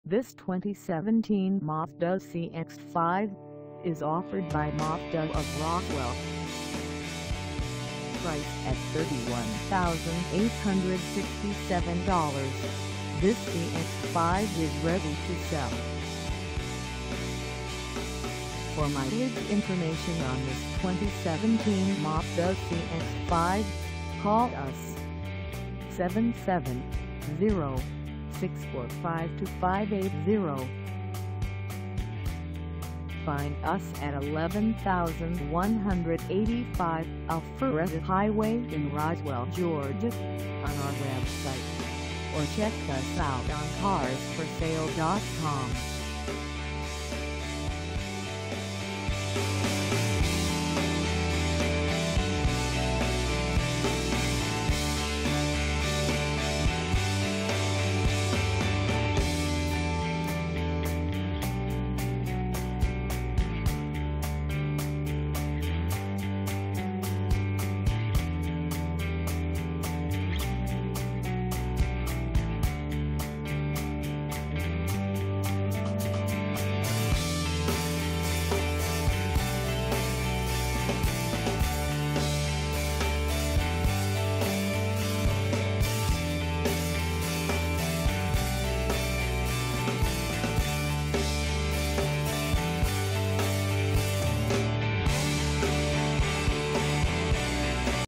2017 Mazda CX-5 Grand Touring AWD 4dr SUV for sale in Roswel
This 2017 Mazda CX-5 Grand Touring AWD 4dr SUV is for sale in Roswell, GA 30076 at Mazda Of Roswell.

Engine: 2.5L I4
Transmission: Automatic 6-Speed
Color: Snowflake White Pearl
Mileage: 0
Contact Mazda Of Roswell 11185 Alpharetta Hwy Roswell, GA 30076 to test drive this 2017 Mazda CX-5 Grand Touring AWD 4dr SUV today.